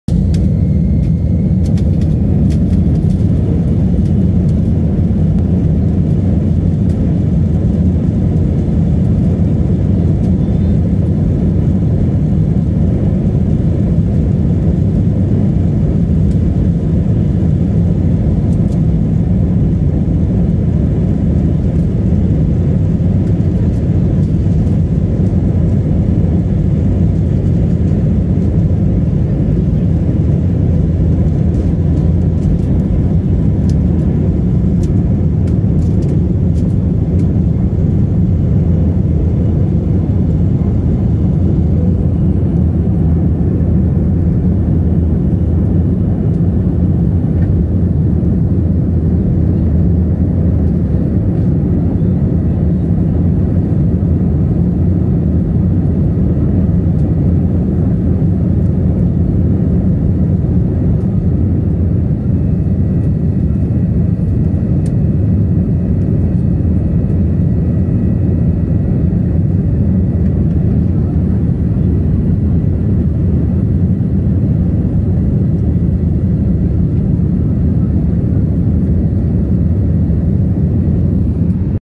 Flughafen Heraklion
Start vom Flughafen Heraklion in Griechenland